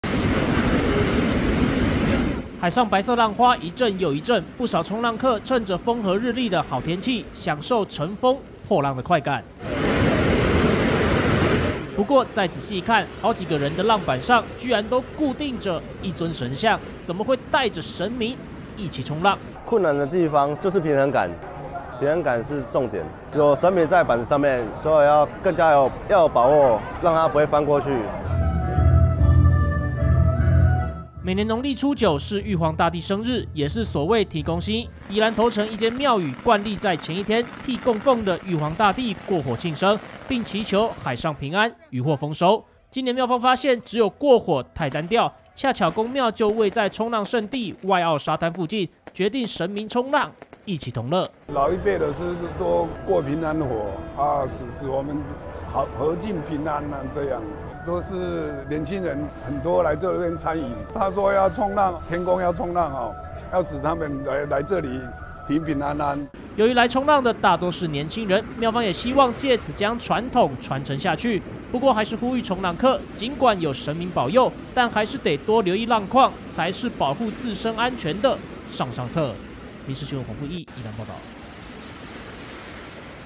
正月初九前一天 為玉皇大帝慶生！ 帶「神明衝浪」祈求豐收－民視新聞
【民視即時新聞】明天是農曆初九，俗稱天公生，宜蘭一間廟宇，按照慣例提前一天，替供奉的玉皇大帝慶生，特別的是今年除了過炭火，還帶神明一起衝浪，祈求海上平安、漁獲豐收。廟方表示會這麼做世，當地衝浪客，大多是年輕人，除了保佑平安外，也希望藉此傳承宮廟傳統。